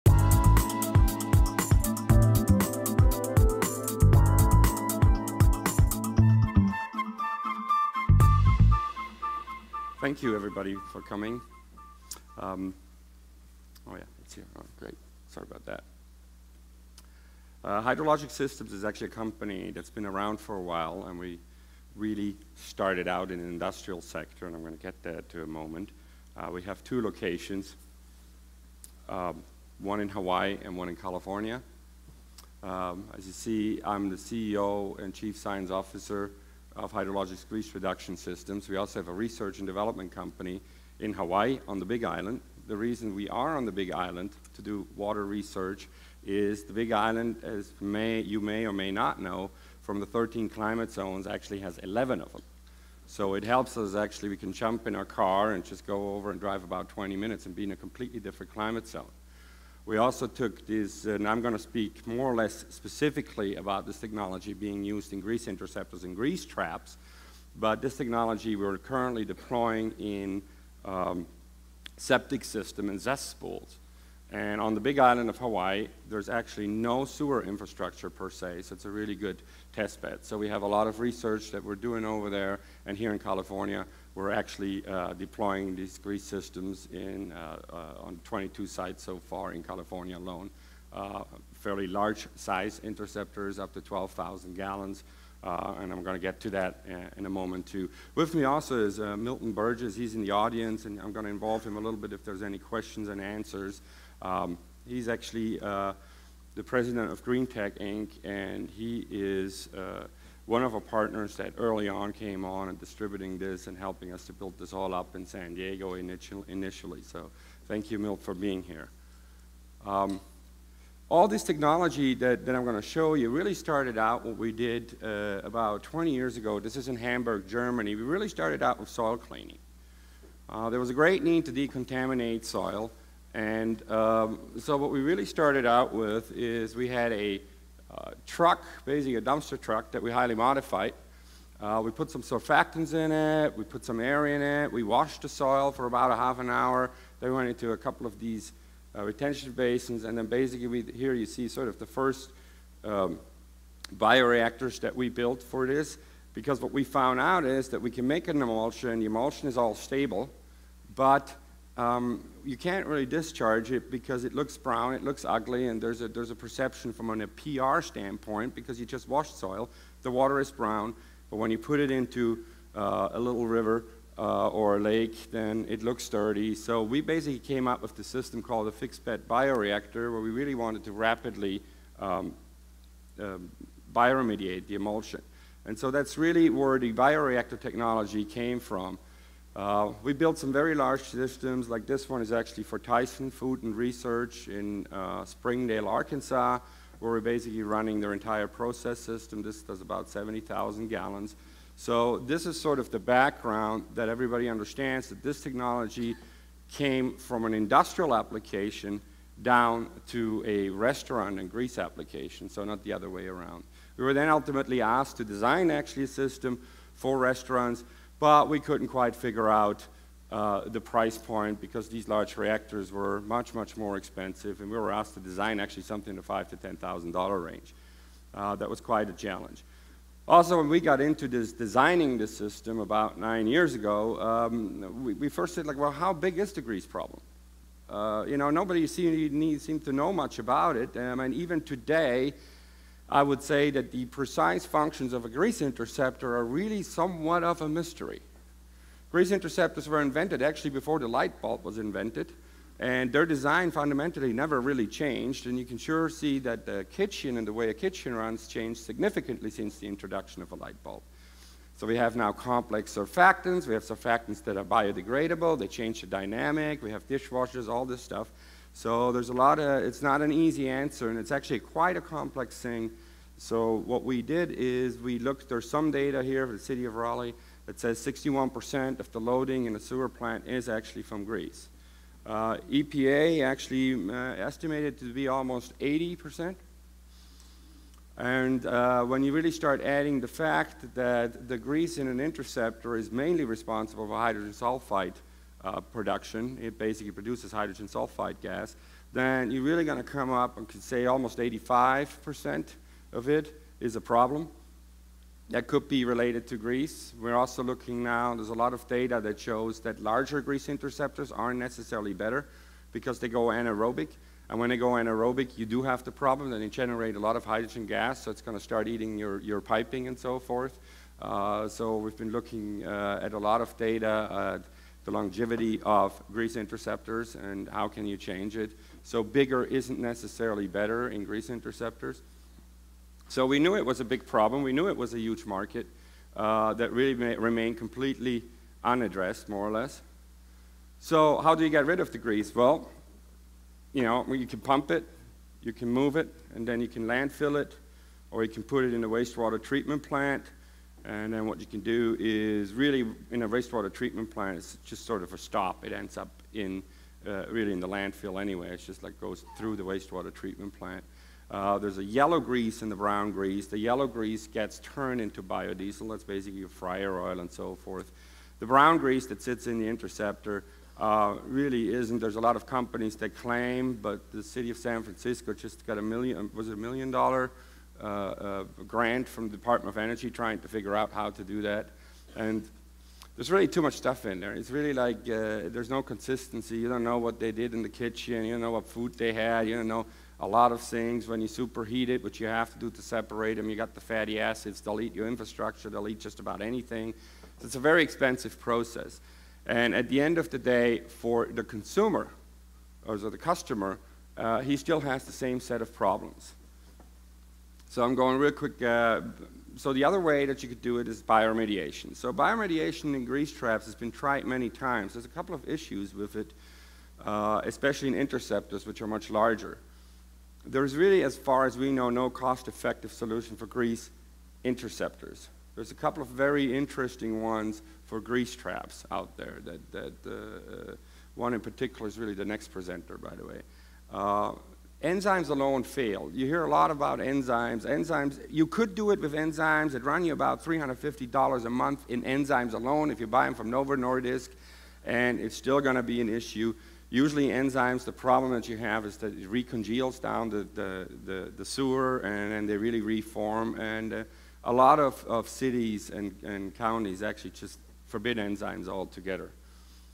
ETS 2010 | Insitu Bioremediation for Grease Interceptors - Part 1
"Insitu Bioremediation for Grease Interceptors" - Dr. Markus Lenger, Hydrologix® Systems, LLC - Part 1 of 4

Hydrologix® Systems LLC has been awarded US Patent No. 7,615,156, Device for In-Situ Bioremediation of Liquid Waste, the first to achieve commercial success with our Hydrologix® Grease Reduction System's treatment of kitchen and sewage wastewater, at the source or "in-situ". This makes the Hydrologix® Grease Reduction System the 'greenest' and most cost effective way today to handle disposal of foul industrial, domestic and agricultural wastewater.

In effect the GRS is a computerized-controlled mini wastewater treatment plant that reduces the potentially hazardous wastewater producing lethal H2S gases, to the harmless by-products of water and CO2. Remote monitoring and live camera feeds help ensure a trouble free performance and optimum reliability. The Hydrologix® GRS's patented technology is a unique system that provides a thriving culture of specially blended microorganisms to instantly digest the liquid waste right in the holding tank, usually a city mandated grease interceptor. The challenge in the past was how to maintain the organisms in such a harsh environment (one that even disintegrates stainless steel) and ensure continuous robust growth, a challenge only Hydrologix® has overcome.

This presents a phenomenal leap forward for the wastewater industry worldwide, which is struggling for more cost effective and sustainable ways to manage the ever-growing waste problem and offload aging city wastewater treatment plants. In San Diego County, installations are going forward through GreenTek, Inc., a California contractor. And in Orange County other installations have been in place for nearly seven years.

This presentation was part of the 2010 International Emerging Technology Symposium, a two-day event that provided participants with an opportunity to view presentations and engage in timely discussions on how the water utility, manufacturing, engineering, and trade industries have found solutions through emerging technologies in the water efficiency, plumbing and mechanical industries.